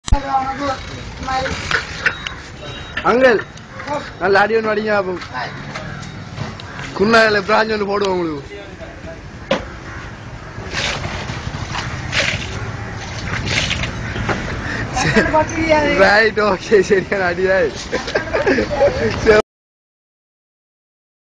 How to cool hot tea / coffee
You can see this in all of south India in road side tea stalls.